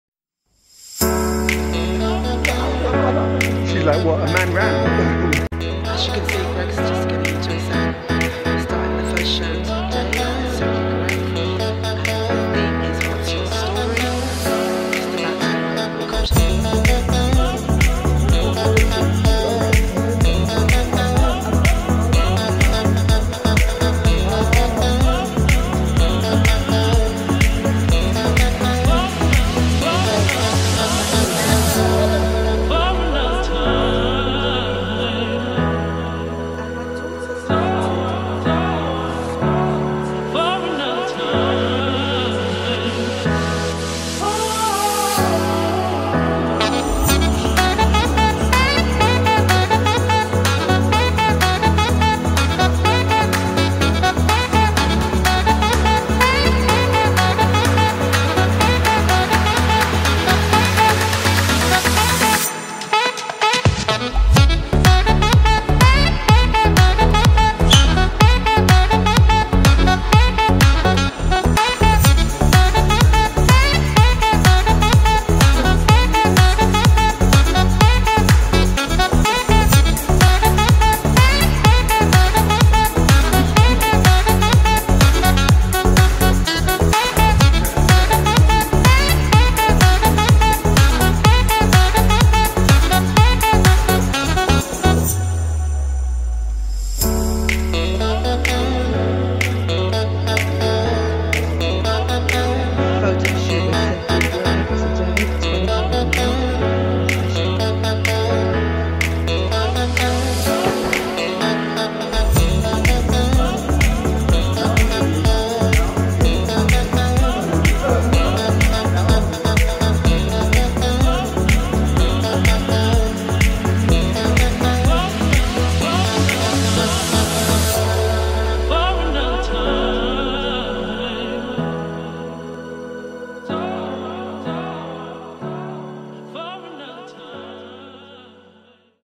The Simply Greg Show - VLOG EP1. | Shana Dawn Lewis Interview | Simply Greg Photo Shoot
Behind the scenes on the set of The Simply Greg Show S3. EP1 with Simply Greg and team, featuring Shana Dawn Lewis, Author of '6 Operations'.